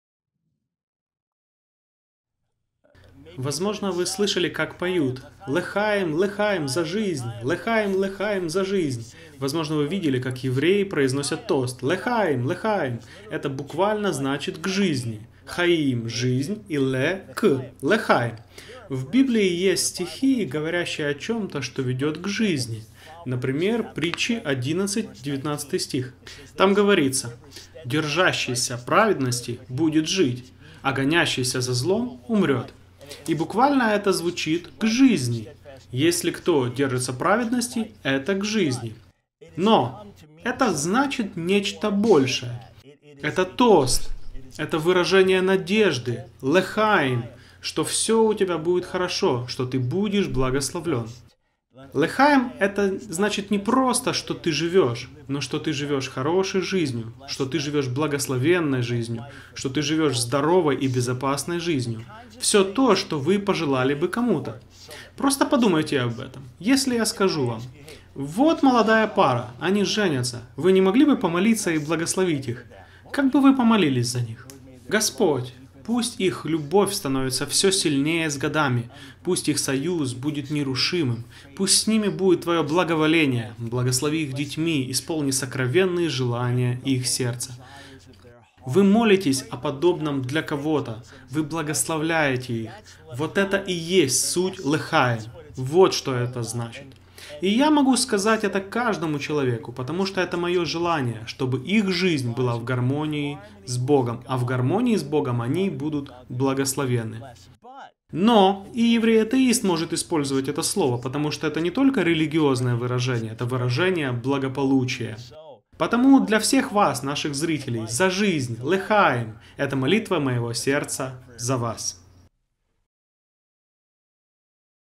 Что значит еврейское слово Лехаим?
Возможно, вы слышали, как евреи поют или произносят тост: "Лехаим! Лехаим!" Но что значит это еврейское слово "Лехаим"?..